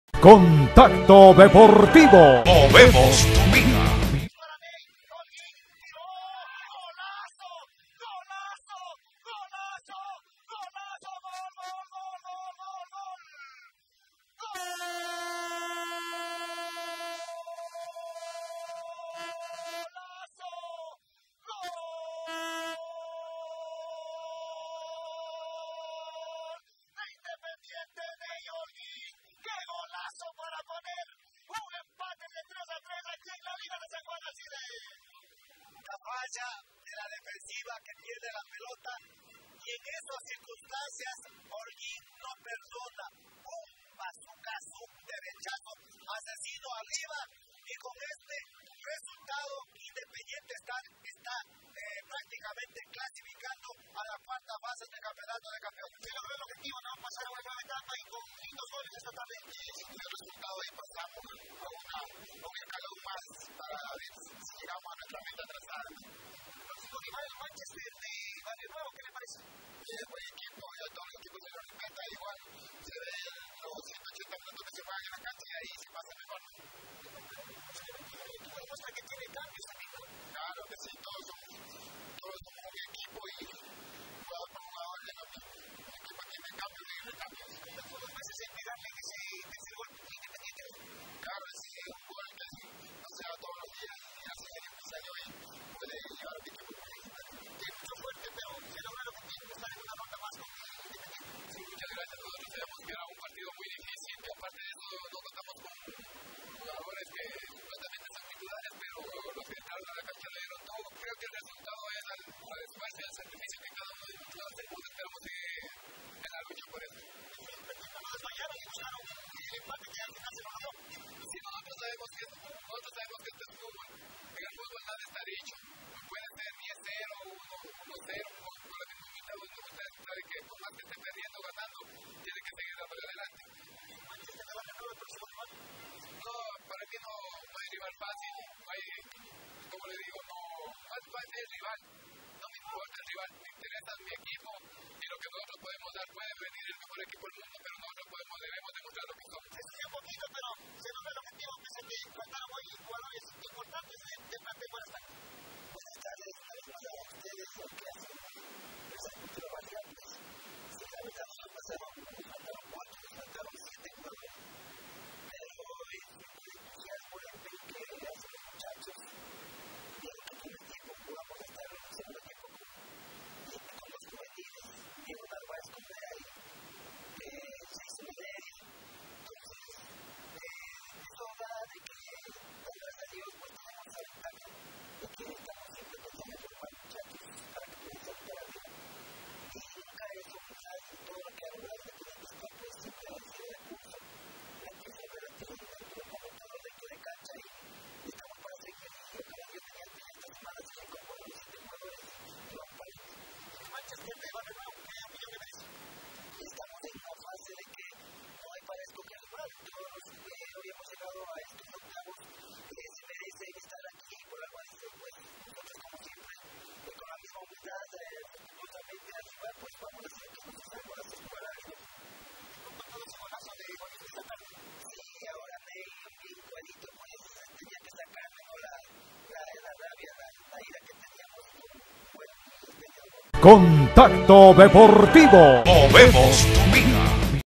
GOL Y ENTREVISTA A INDEPENDIENTE STAR
Con el el empate a 3 goles por bando Independiente Star de Liga la Magdalena dejo en el camino al Nacional de Liga San Juan y su proximó ribal será Manchester de Barrio Nuevo